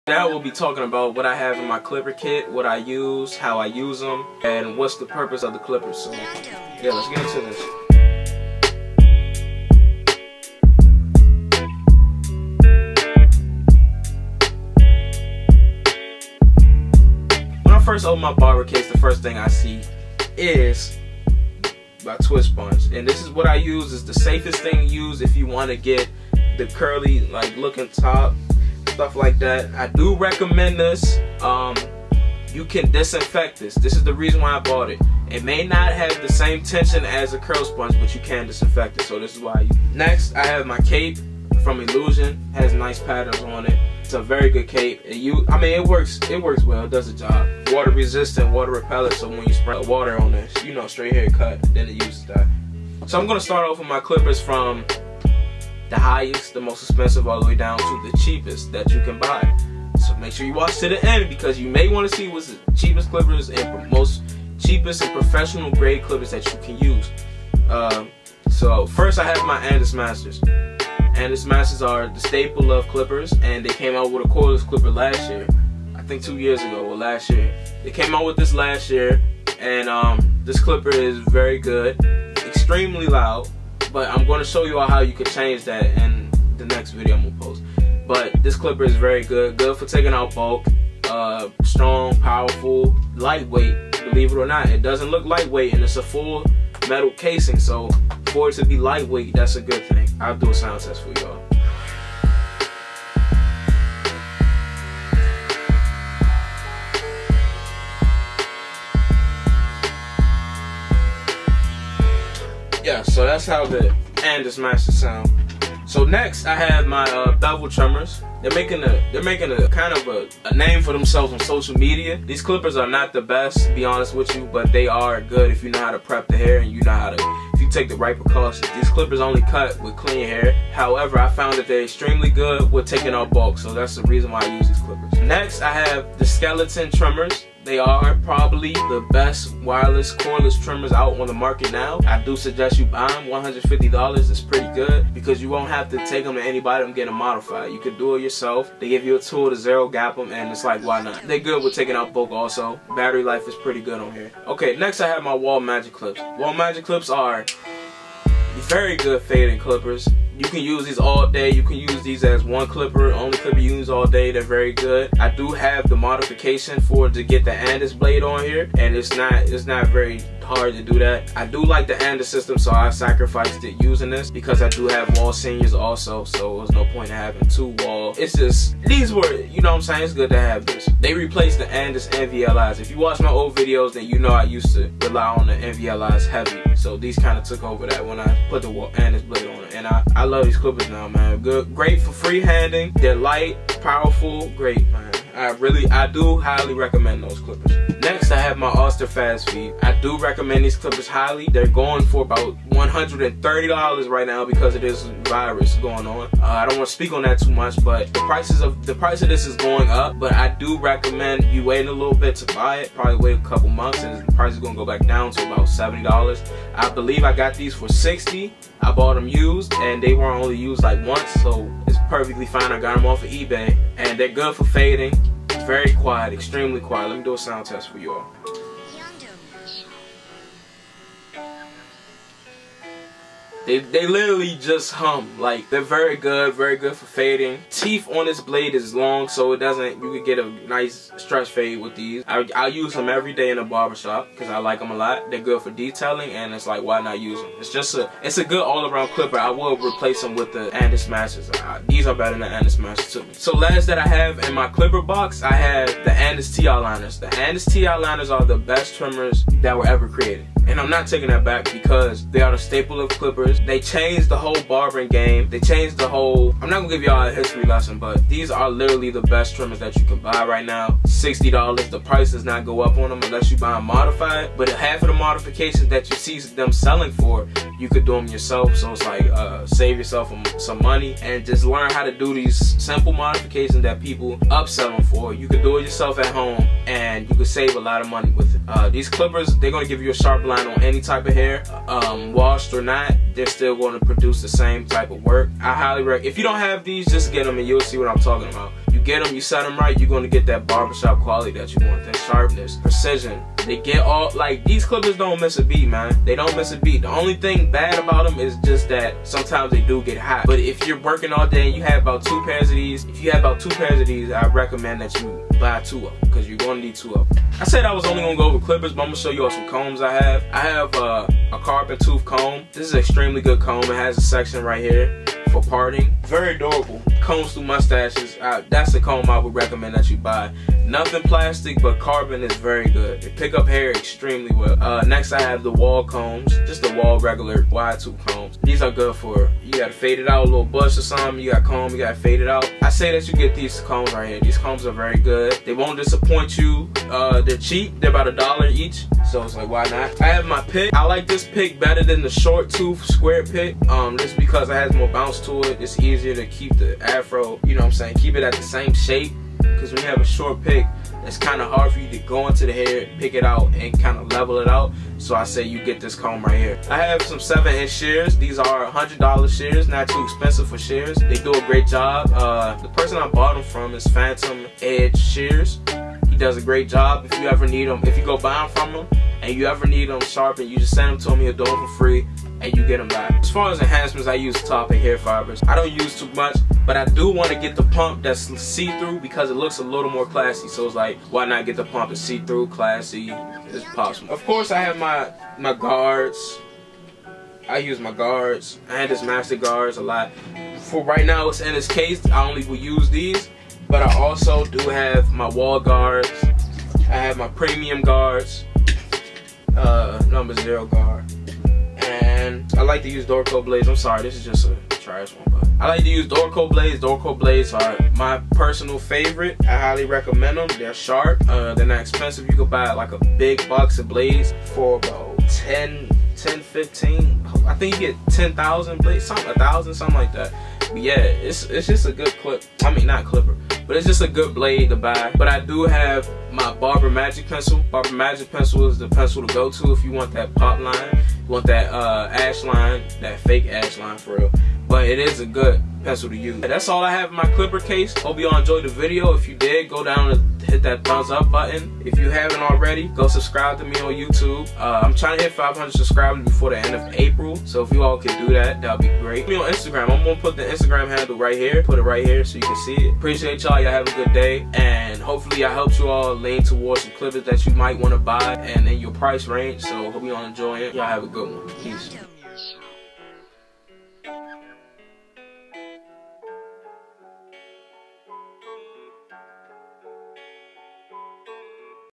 What's in My Barber Case ??
In this video I explain the tools I use ! These tools are good and not too expensive at the most! I explain some of the high quality and some of the low quality clippers!!